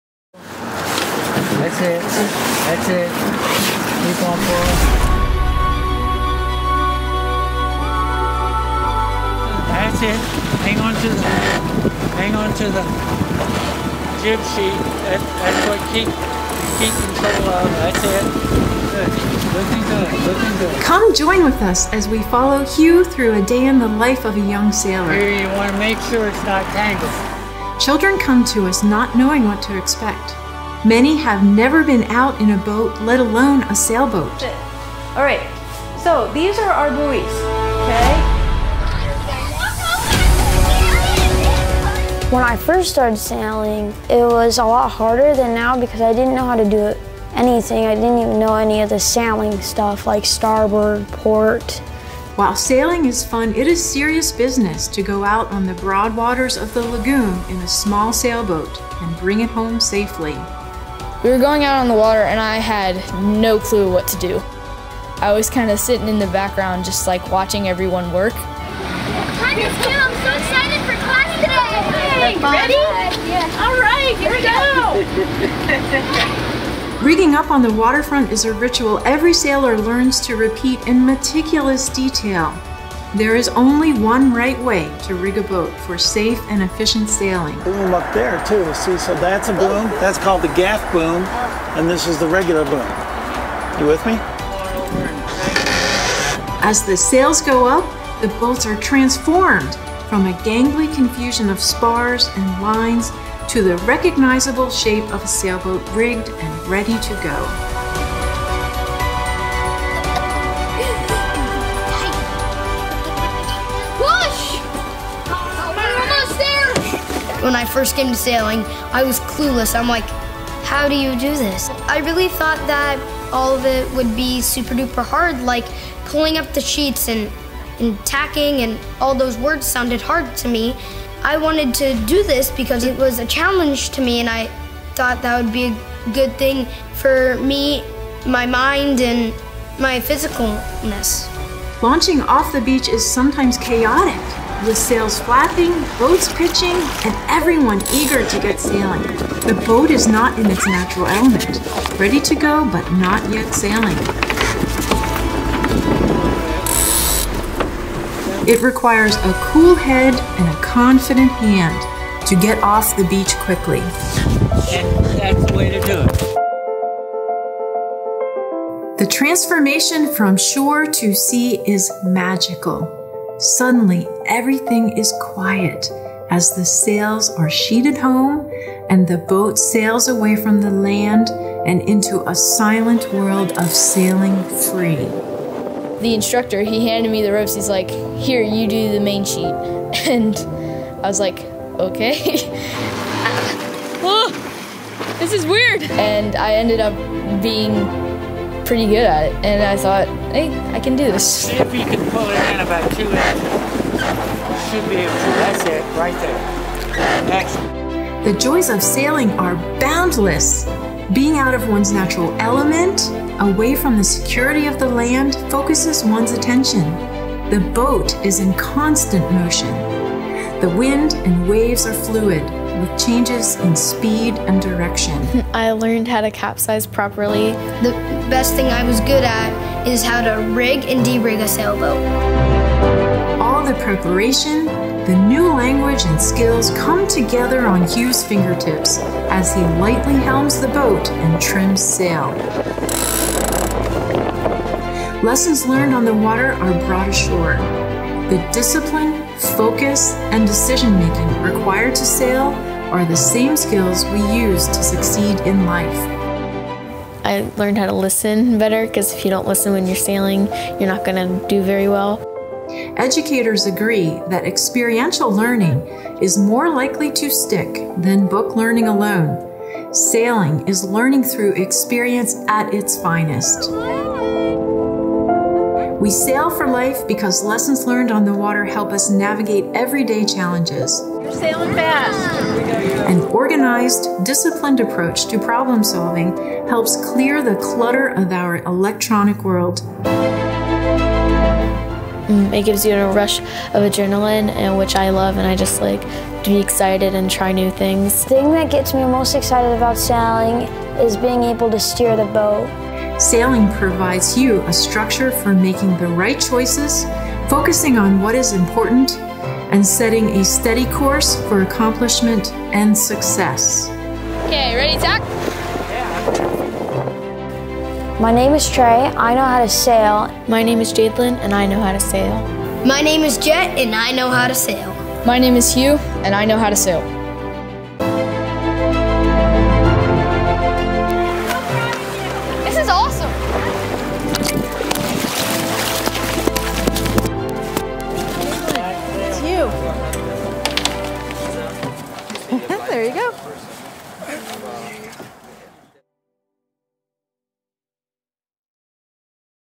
Youth Sailing Foundation of Indian River County - A Day in the Life
See why we do what we do.  This beautiful and artistically crafted video shows the impact that sailing can have on a youngster.  While it features our after school squadron program, it is indicative of all YSF programs (most of which are offered free of charge) which engage and empower local kids who would otherwise be denied the opportunity to experience the thrills and challenges of sailing.